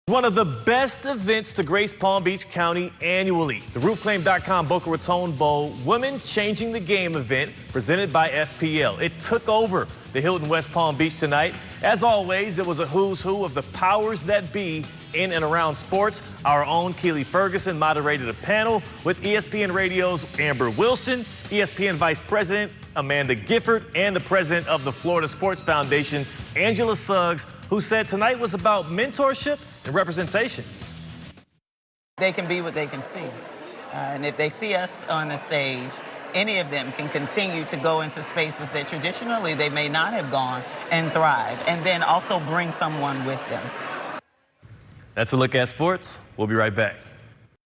Boca Raton Bowl Women Changing the Game event returns to West Palm Beach
The Boca Raton Bowl Women Changing the Game event returns to West Palm Beach ahead of the 10th annual postseason game.